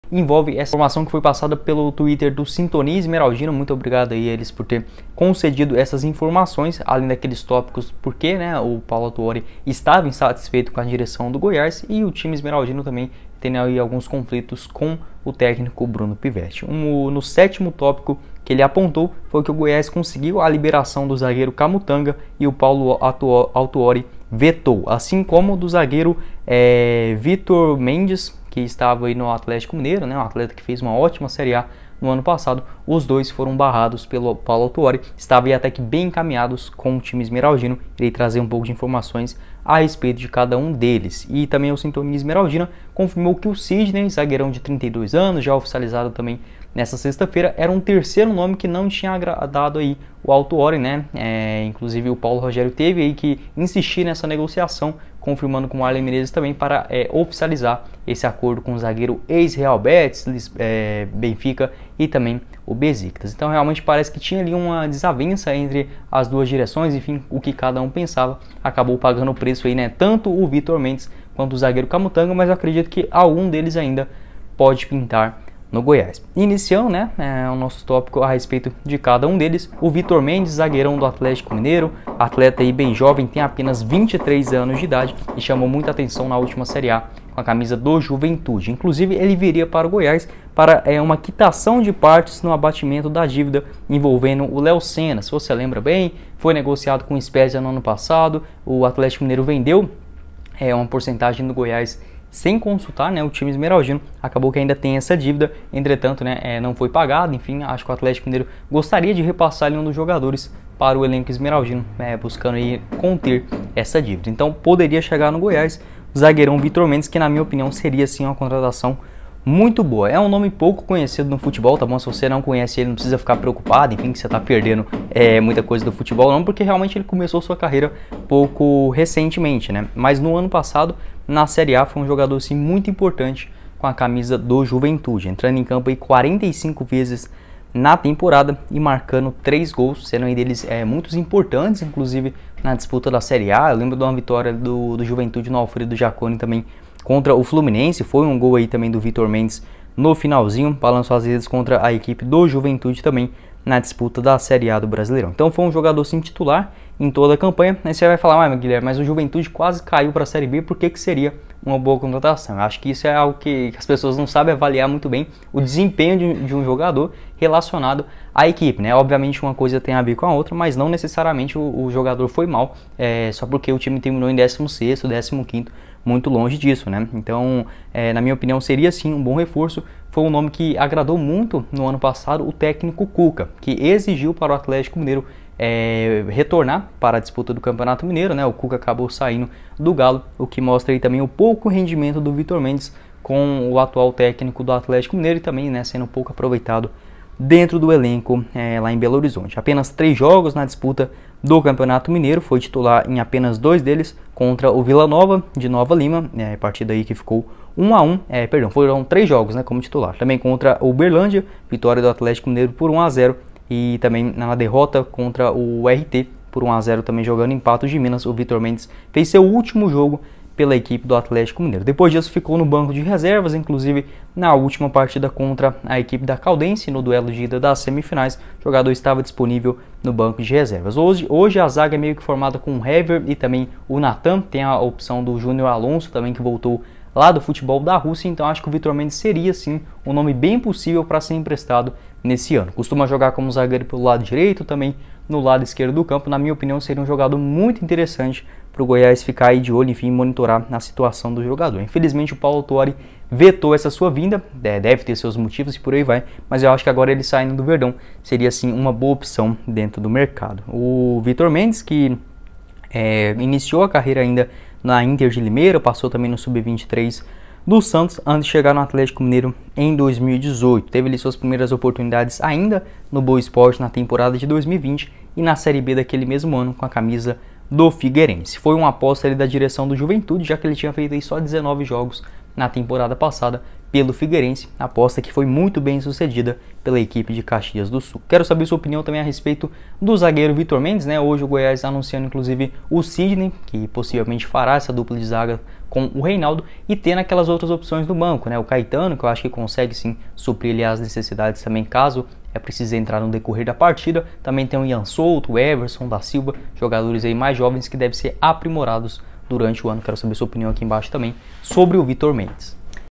💚🔥 Vitor Mendes Ainda Pode Chegar No Goiás Em 2022?Por Qual Motivo Autuori Barrou?Atlético Libera?
🔵🇧🇷 Faça já o seu cadastro na Br4Bet!

🤔 Seria boa contratação para o Verdão?

✅ Goiás poderia tentar buscar o Vitor Mendes novamente?

🔥💚 Siga a nossa página no Instagram: @GoiasNews1943